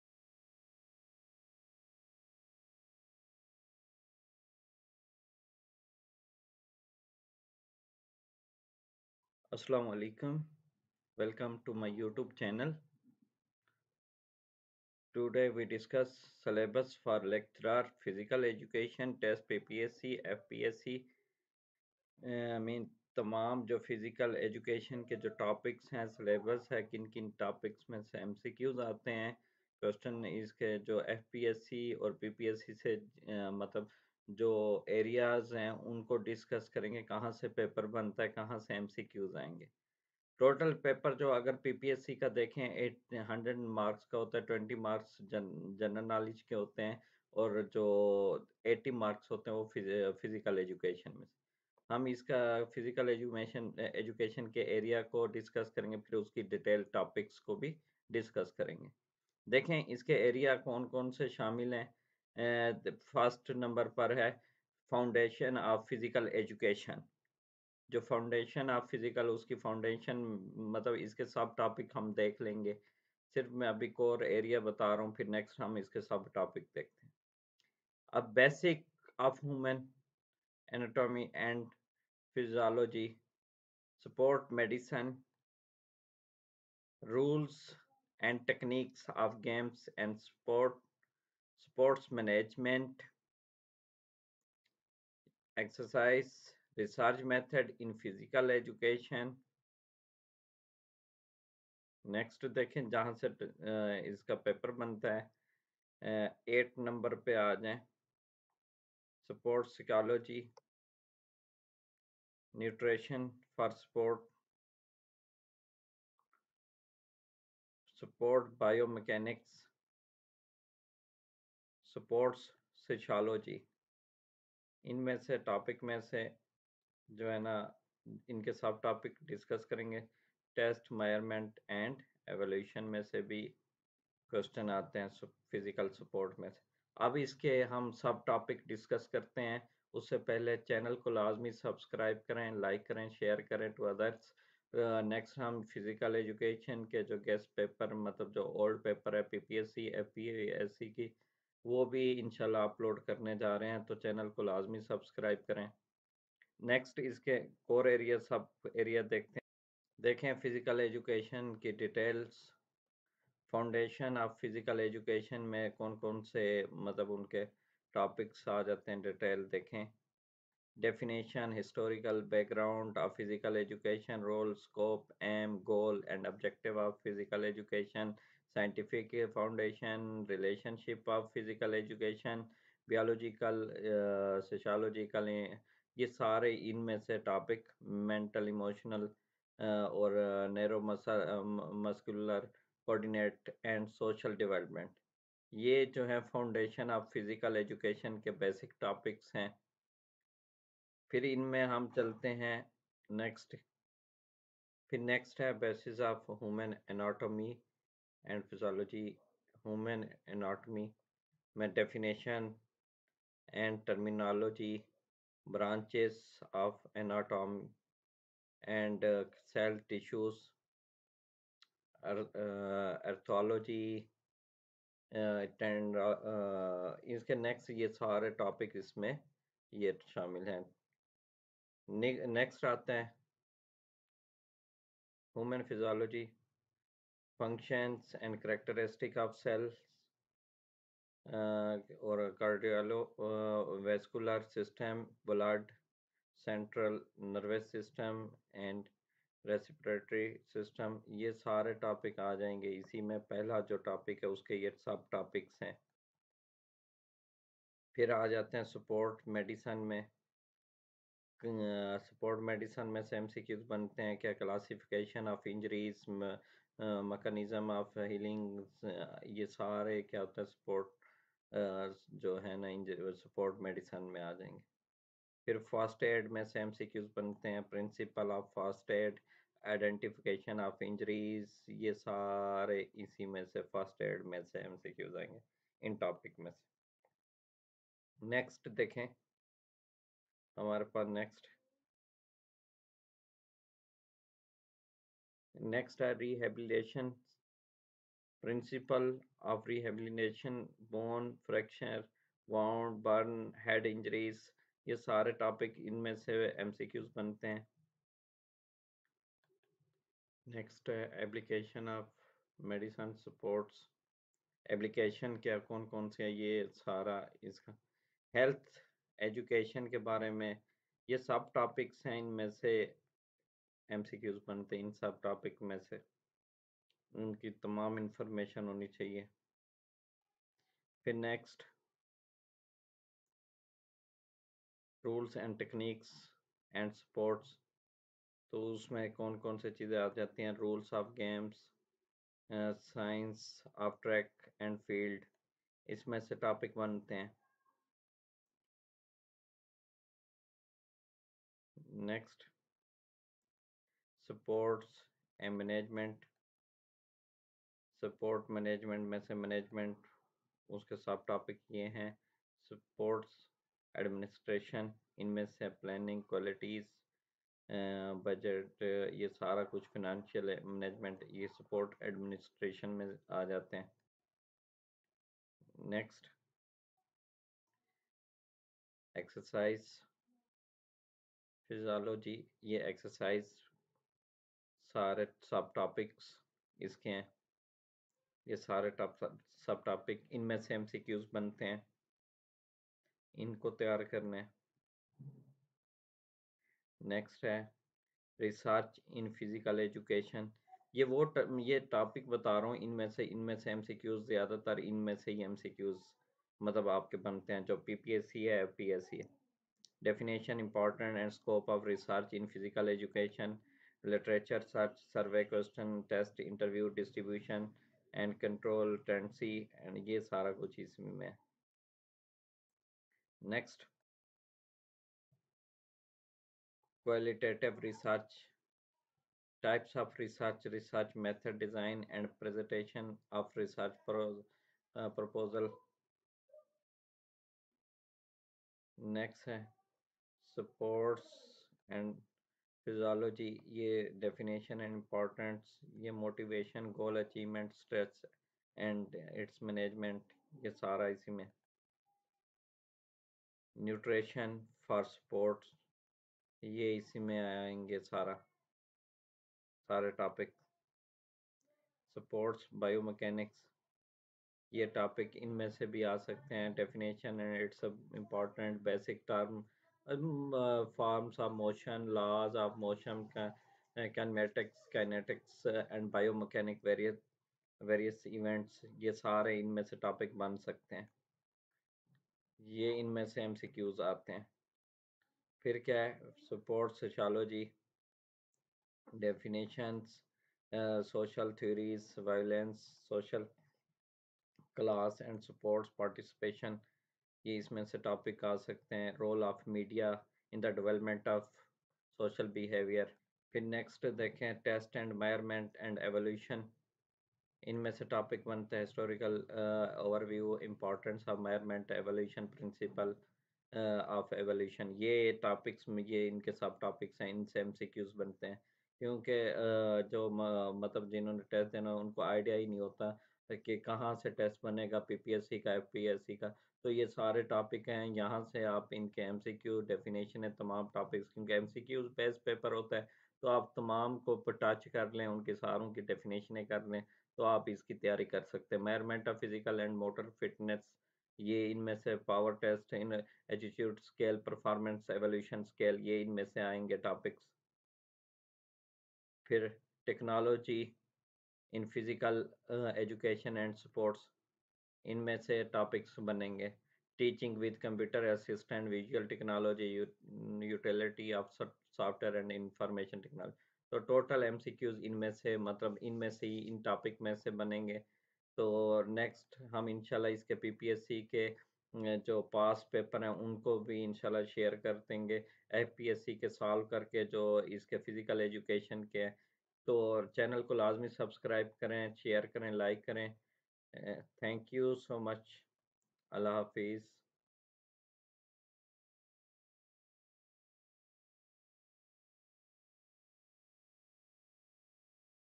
How to prepare PPSC Physical Education Test | Lecturer | Assistant Professor | FPSC | NTS
@naveedjamal786  In this I give full guideline regarding Physical Education Test of PPSC, FPSC , BPSC , KPSC, NTS etc